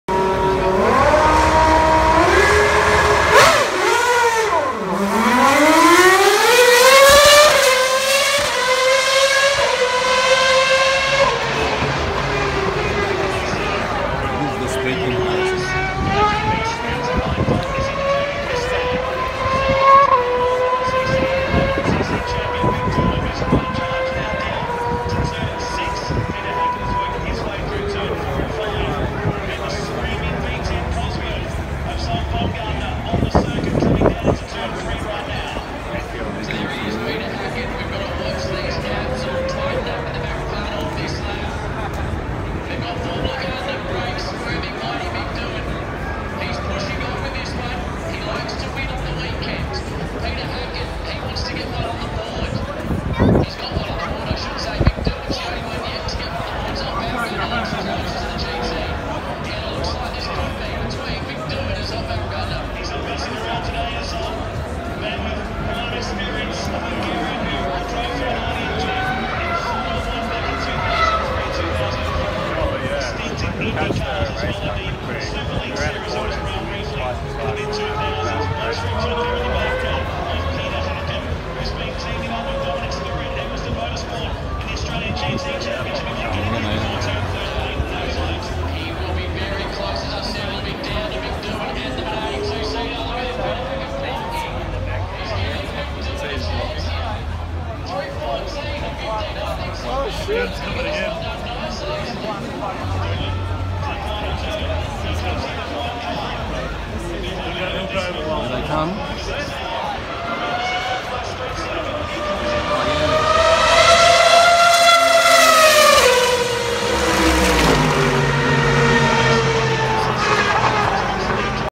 Australian Grand Prix F1 2018 - V10 Minardi Speed Comparison Event
This was a race to show difference between 3 cars - the one fans were interested in was the two seater V10 Minardi Cosworth.
The sound was from this car was the loudest of all cars during the whole event - F18 fly over may have topped it!
Video starting at the V10 Minardi start which was around 15 seconds or so after the Mercedes AMG started.
Let you guess who wins! Enjoy.